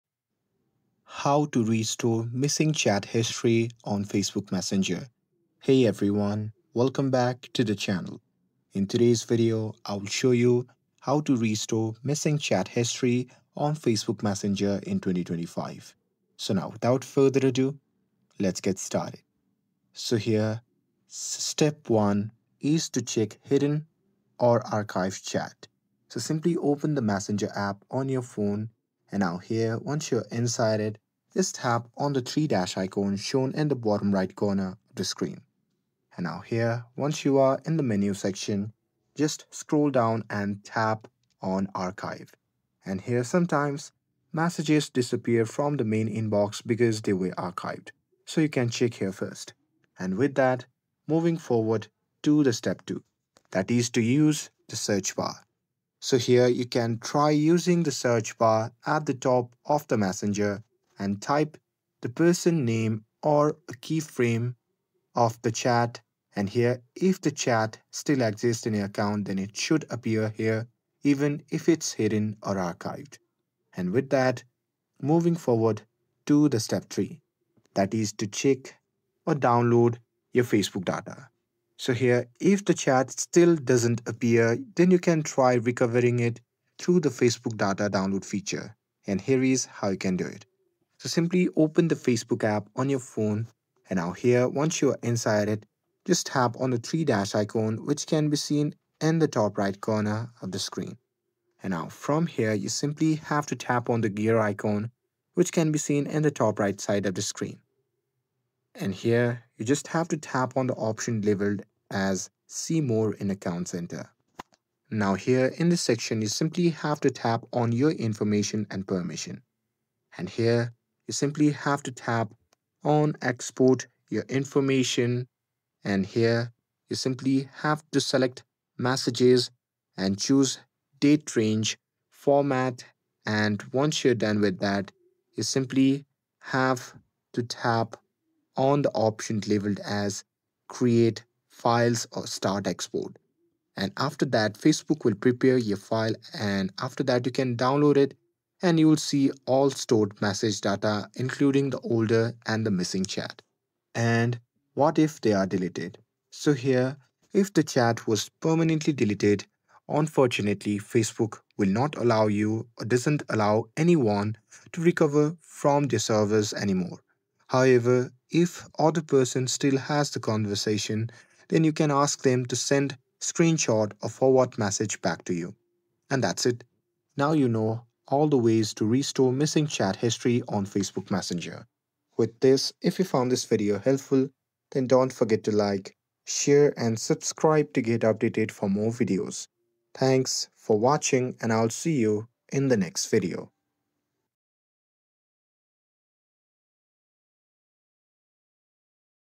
How to Restore Missing Chat History on Facebook Messenger (2025)
Lost your Facebook Messenger chats? 💬 Don’t worry — this step-by-step 2025 guide will show you exactly how to restore missing chat history on Facebook Messenger, whether you’re using Android, iPhone, or desktop. Recover your messages safely and get your conversations back in minutes!

What You’ll Learn in This Video:

✅ How to check archived and hidden chats on Messenger
✅ How to recover deleted messages using Facebook data download
✅ Tips to sync your chat history across devices
✅ How to prevent losing messages in the future

Whether you accidentally deleted chats, switched phones, or lost messages after an update, this video will help you restore your Messenger conversations quickly and securely.

📌 Chapters:
⏱ 0:00 - Introduction
⏱ 0:15 - How To Restore Missing Chat History on Facebook Messenger